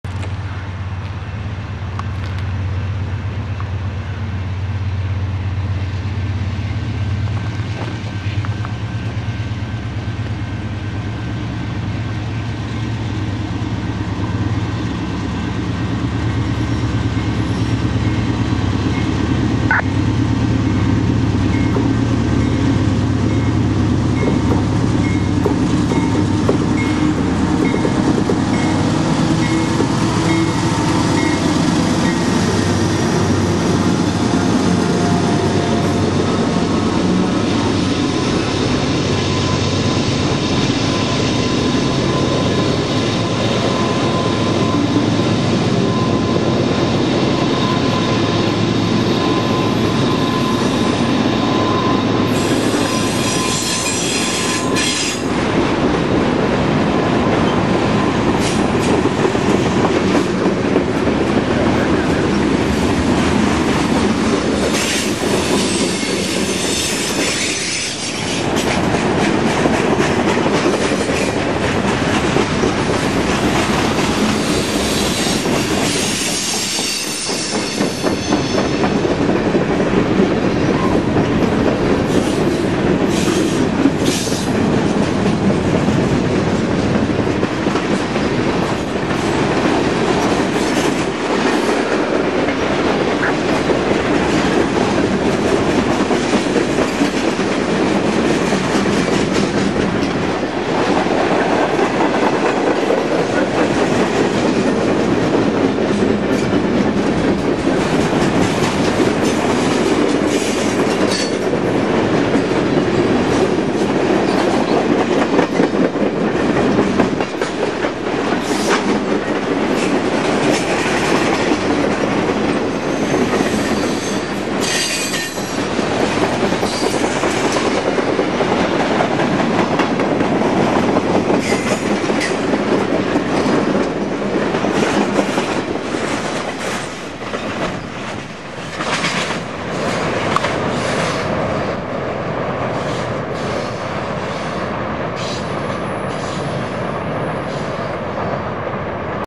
Cambridge 20.06.06 1786
CP #241 leaves Galt for greener pastures.